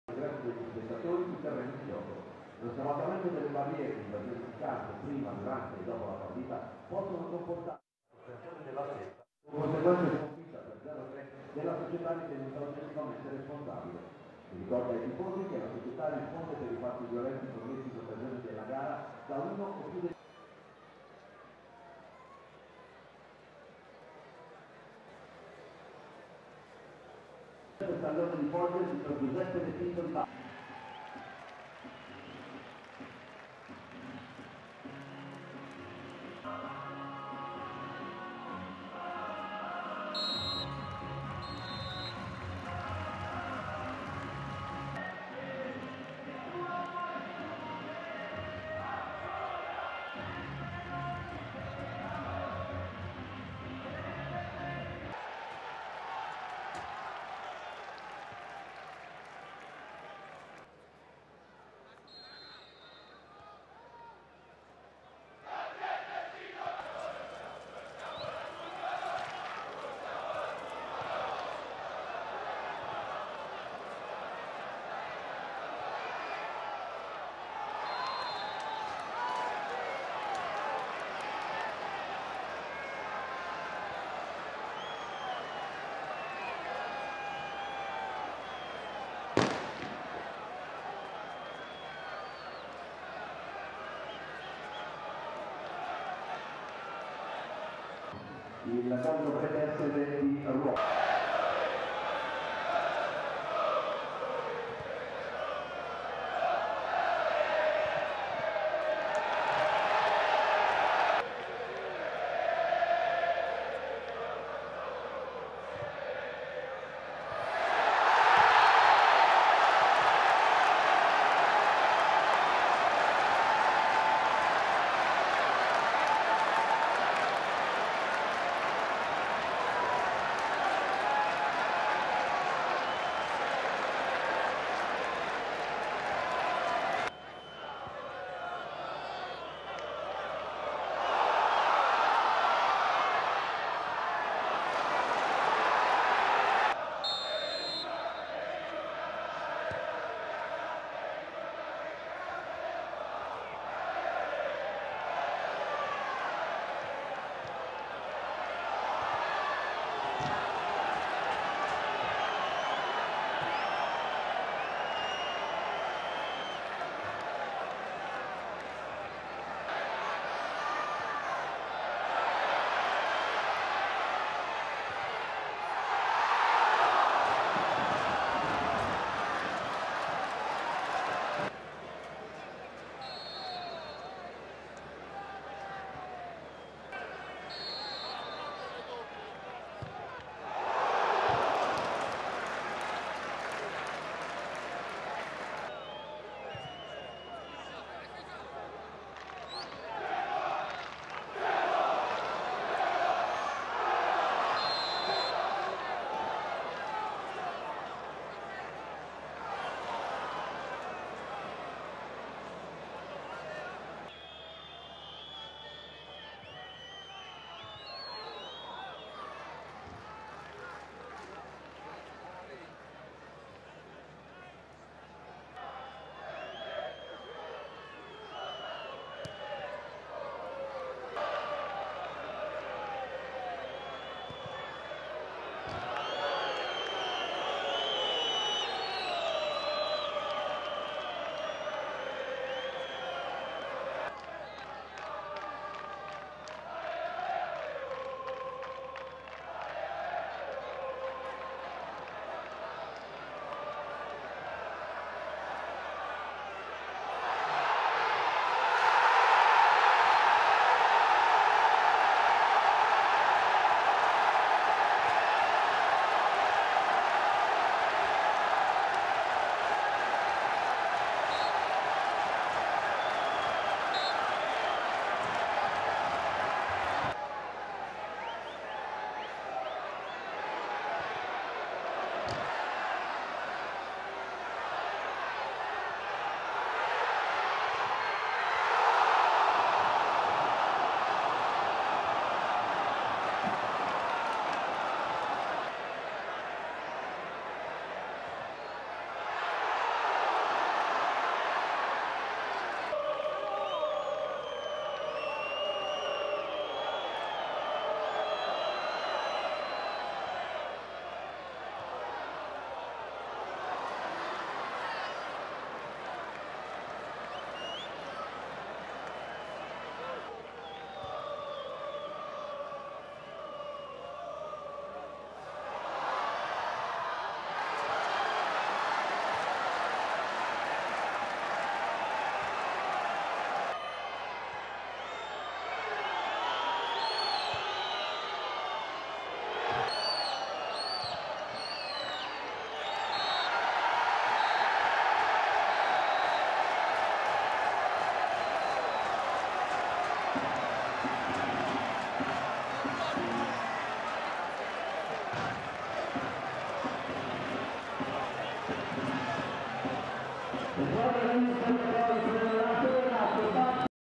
ANCONA - Albinoleffe 2-1 gol con effetti stadio
senza telecronaca